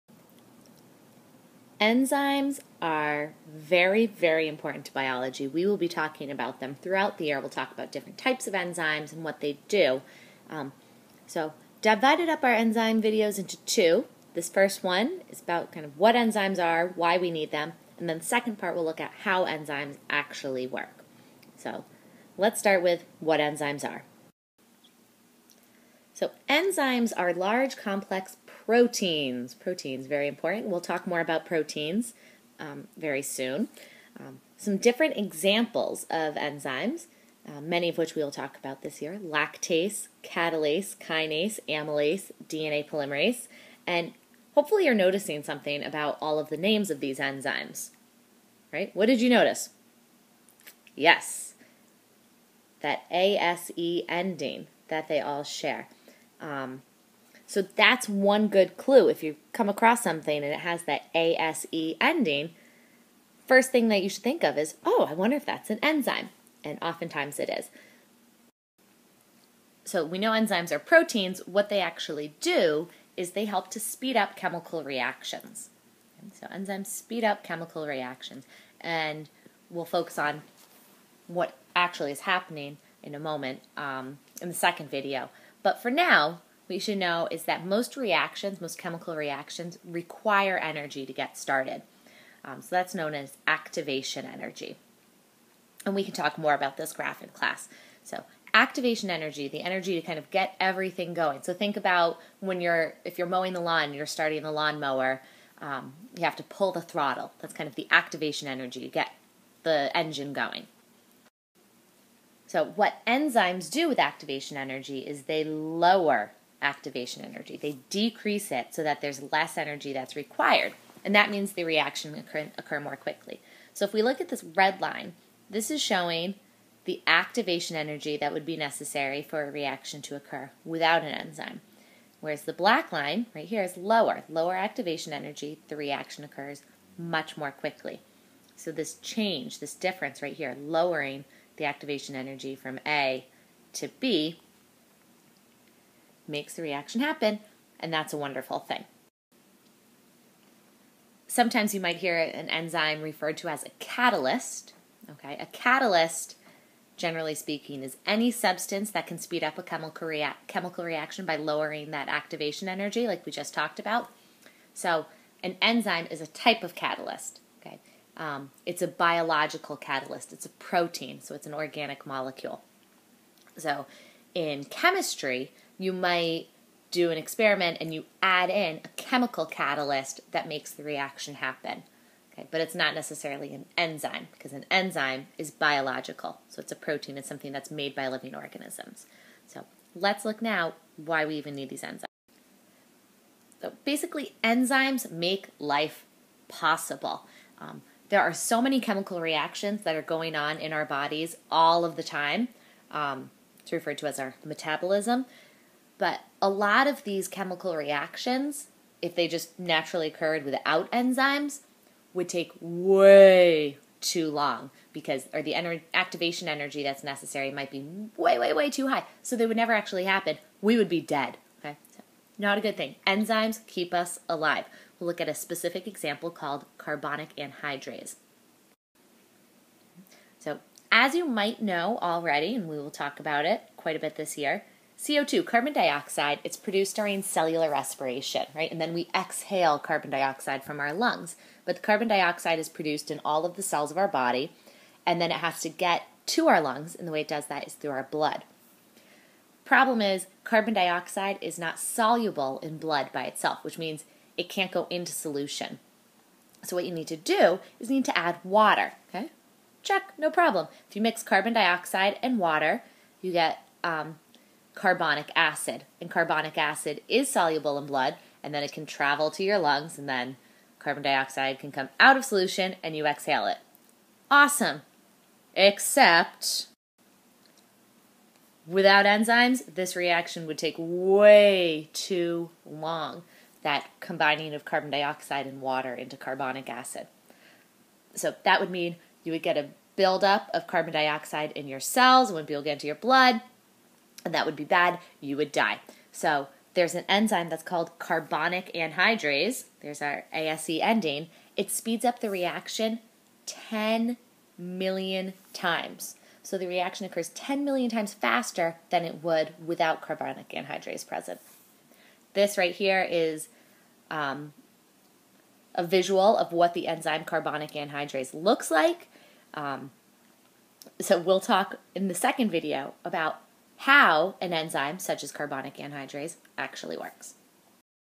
Enzymes Part 1 - What & Why
You should include the following in your notes:
-Definition and examples of enzymes (-ase ending!)
-Function of enzymes
-Why enzymes are necessary